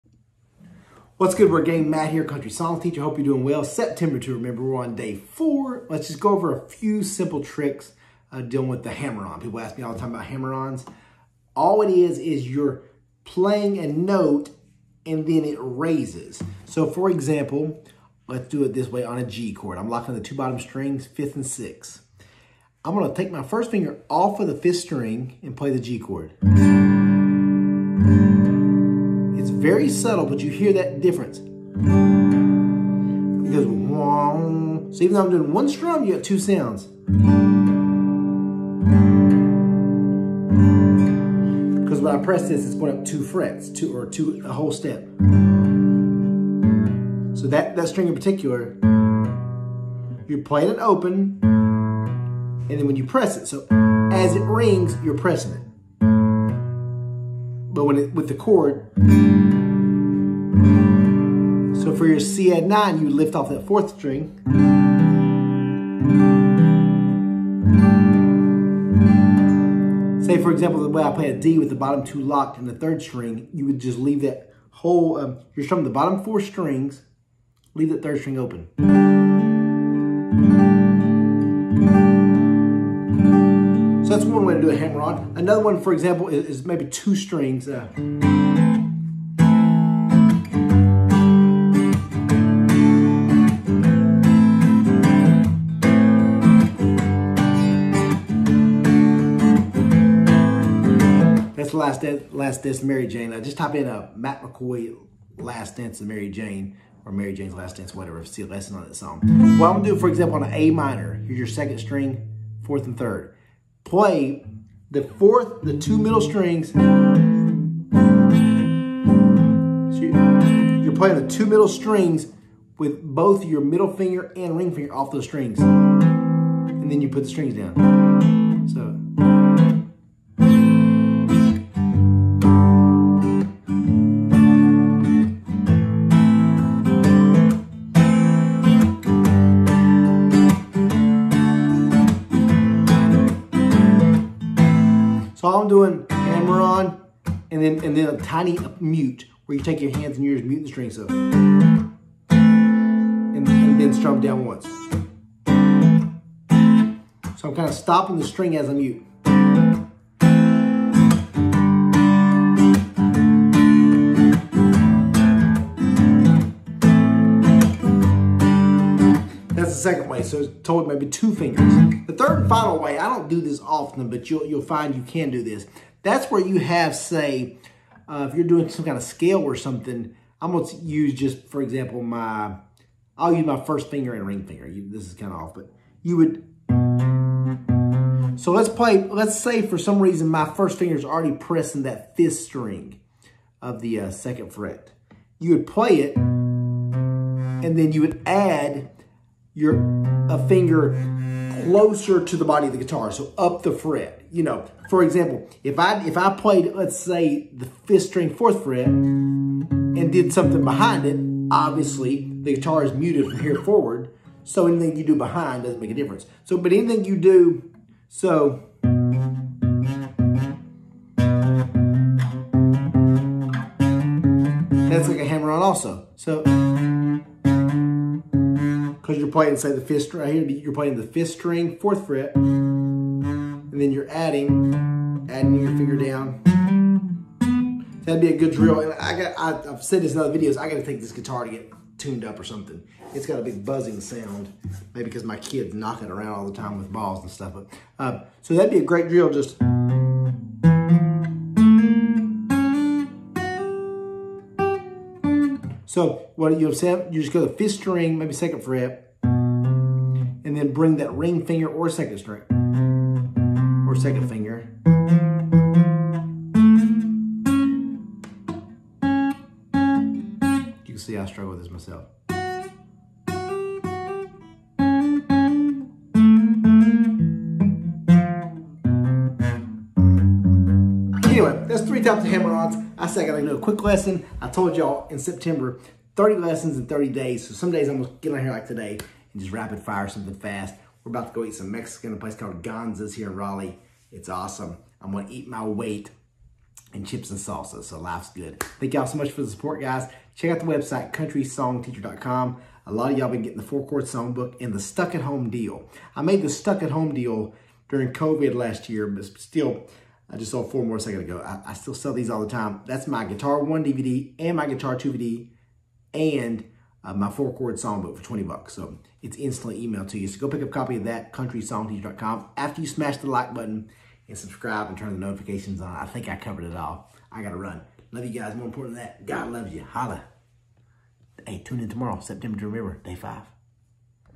3 Hammer On Methods for Acoustic Guitar
Try these 3 hammer on techniques to spice up your acoustic guitar playing.

*Beginner Guitar 1 & 2 DVD (Available in Hard & Digital Copies & T Shirts on site) 💿💿

God bless!
Matt McCoy
-Country Song Teacher